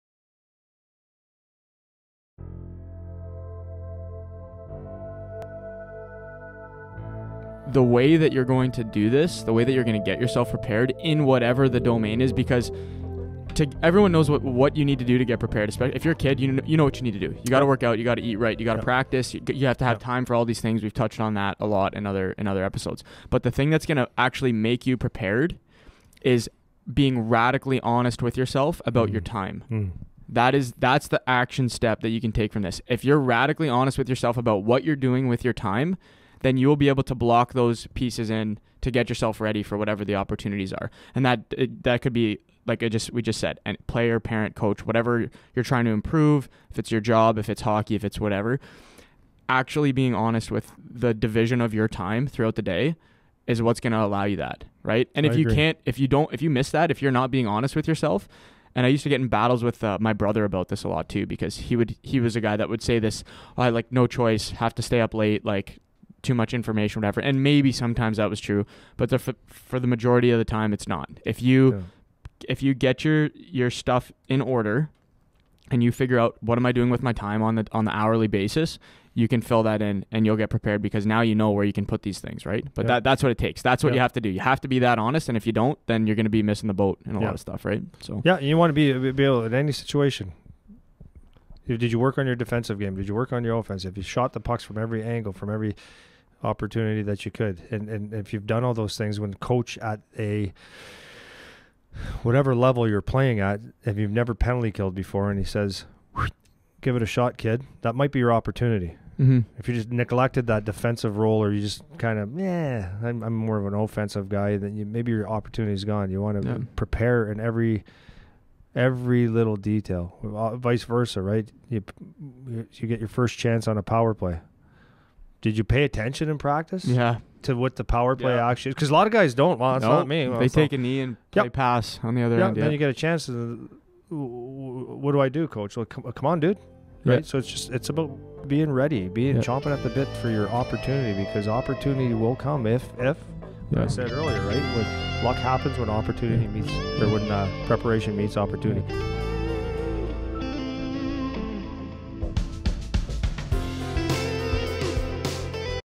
The #1 character trait needed to ENSURE you are ALWAYS PREPARED | Coach Andy
Clip from PowerTech Development Podcast Episode 18 covering preparedness in hockey.
Struggling to ensure you're ready to excel when an opportunity arises? Coach Andy and Eric outline the KEY SKILL needed to increase your preparedness and ability to perform!

POWERTECH ONLINE DEVELOPMENT WEBSITE IS NOW LIVE!